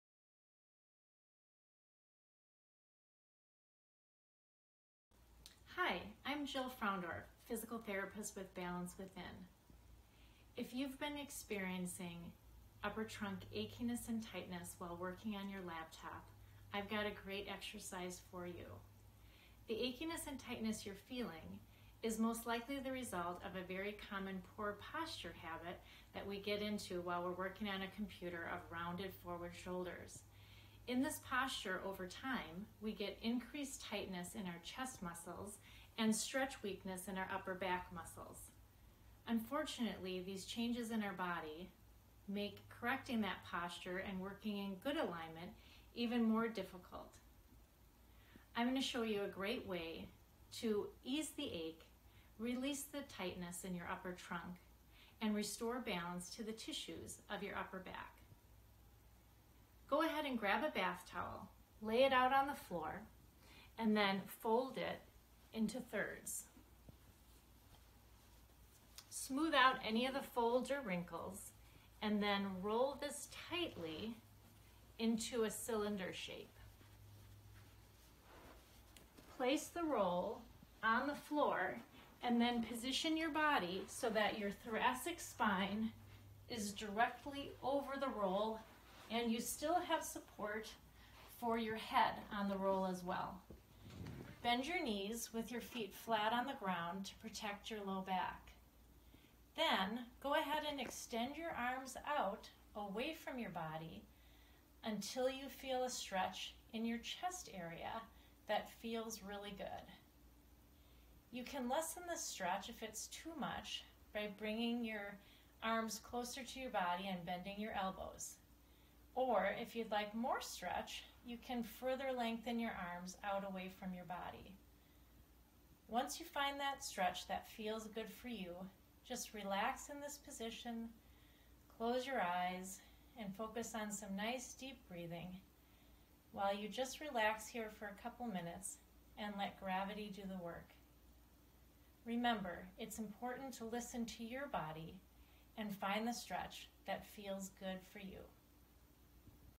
Sitting Posture and Neck Pain
Your sitting posture could be causing your neck pain.  Watch Dr. Jill explain a simple exercise to try at home to help open your chest and upper back muscles to ease your neck pain.  All you need is a bath towel.  For more information on neck pain relief,